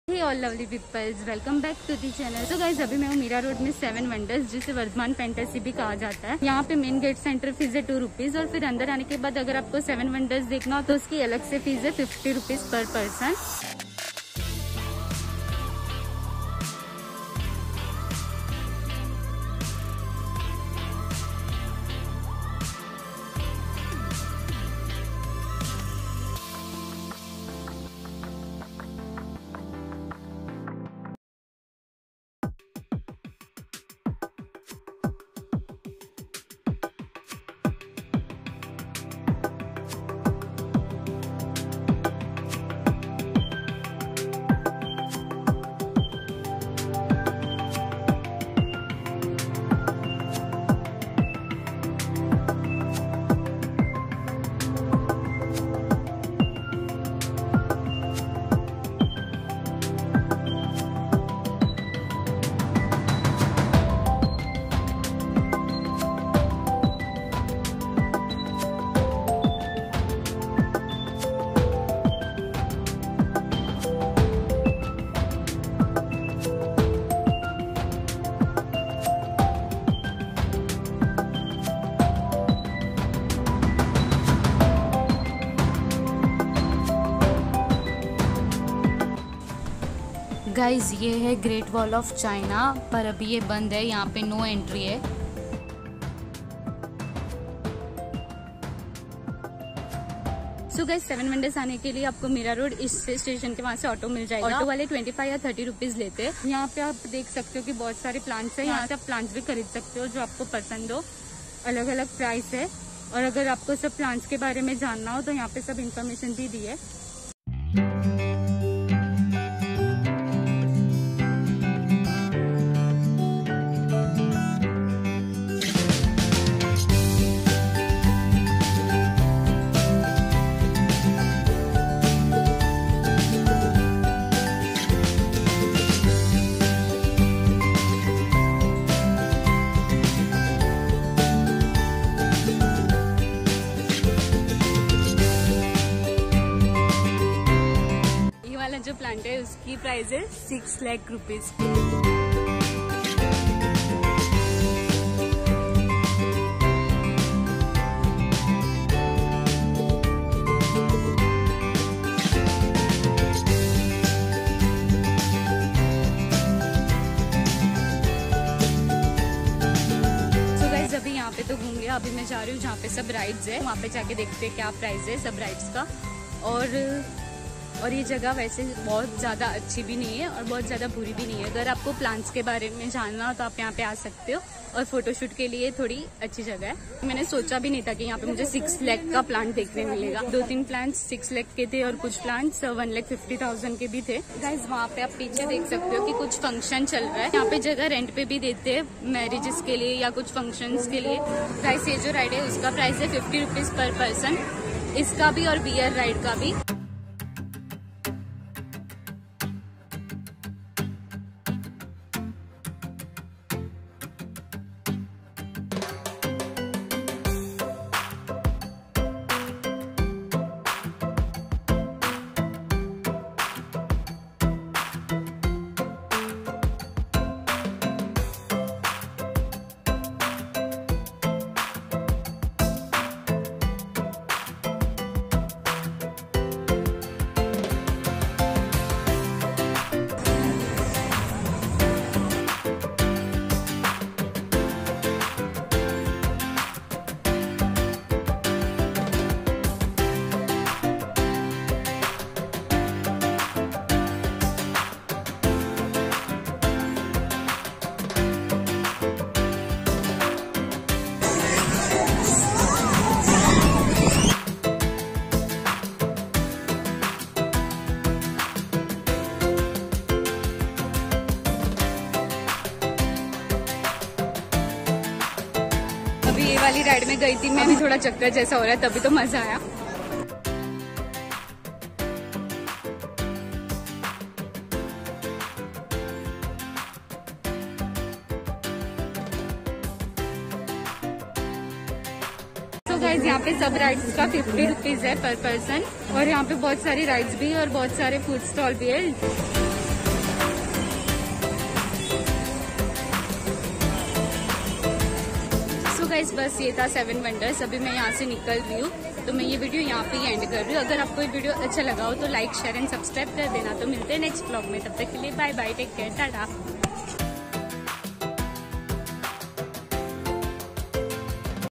Seven Wonders In Mira road | Vardhman Fantasy Amusement Park Reopened
Hello Viewers, Welcome To Your Channel Vlogger Trushali. This vlog is about seven wonders in mira road also known as vardhman fantasy amusement park. in this vlog you will get to know about entry fees of seven wonders garden, how to reach and other information.
Mira road station is situated in the western line of mumbai suburban railway network. One can easily get public transport to reach seven wonders garden. entry fees of vardhman fantasy from main gate is 2 rs per person. after coming inside one side there are different different rides such as giant wheel, break dance, free fall any many others. rides costs are 50 rs per person for one ride. in another side there is miniature replicas of seven wonders of the world which is taj mahal, statue of liberty, eiffel tower, tower of pisa, great wall of china, christ the redeemer, the colosseum and chichen itza. one can also explore make my garden which is plant farm inside of vardhman fantasy. you will get knowledge about plant farming also you can buy plants from here.
Timing of vardhman fantasy park is 9 am to 9 pm. I will suggest to go in evening time because mostly all ride starts in evening. If you have any question you can ask in comment section.

I hope you'll like this vardhman fantasy vlog. for more travel vlogs visit on my channel.

Share All Your Thoughts That You Want Me To Add In The Next Videos.